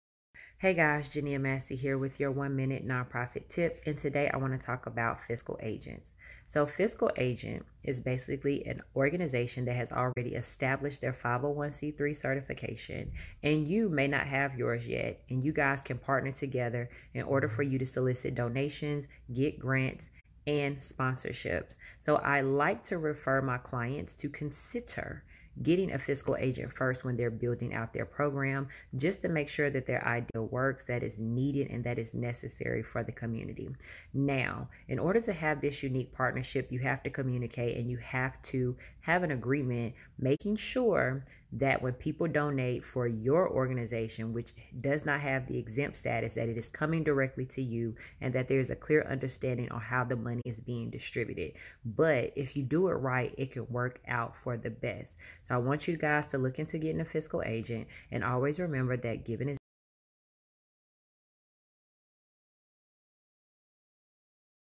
When to get a Fiscal Agent?
How you can begin fundraising without your 501c3 confirmation or any documentation that you’re a “legit” non-profit??

 👉🏽 Get a Fiscal Agent!

💰 Fiscal Agents are existing non profits with their 501c3 confirmation or “legit” tax exempt entities. These orgs can act as a sponsor on behalf of your budding non-profit.

👀 Think I’m lying?

💰 Fiscal agents can also collect funds on your behalf and your supporters will still be able to write them off on their taxes. Yes, I know.. we love it here too. 💁🏽‍♀️

Any questions? Drop them in the comments below. 👇🏾